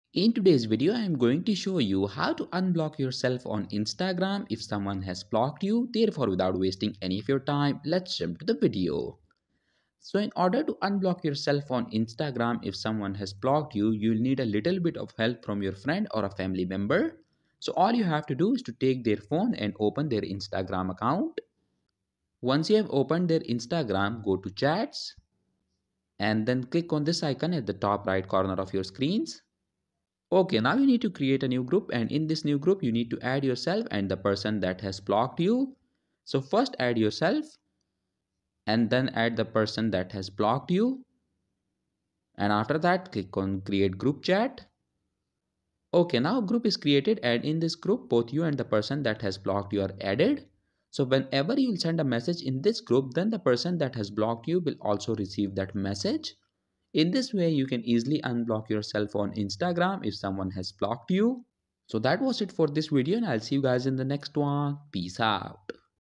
How to Unblock Yourself on Instagram in 2023 if Someone has Blocked You? Get Unblocked on Instagram
Has someone blocked you on instagram and now you want to get yourself unblocked then this video is for you. Because in this video I will show you a simple method that you can use to get yourself unblocked on instagram if someone has blocked you. So sit back and learn how to unblock yourself on instagram in 2023 if someone has blocked you.